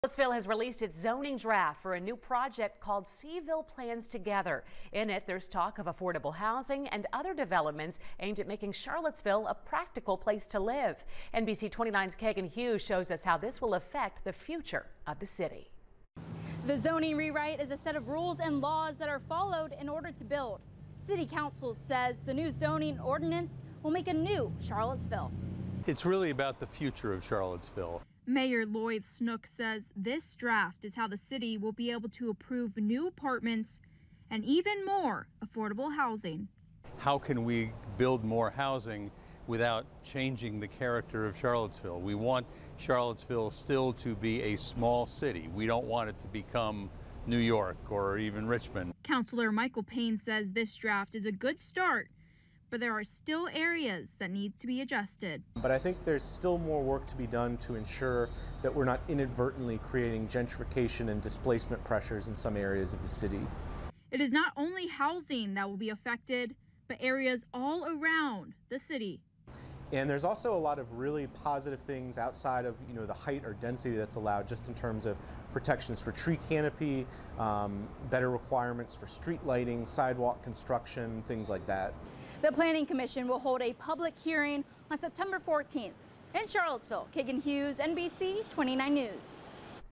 Charlottesville releases zoning draft for Cville Plans Together project
Mayor Lloyd Snook says the draft is how the city will be able to approve new apartments and more affordable housing.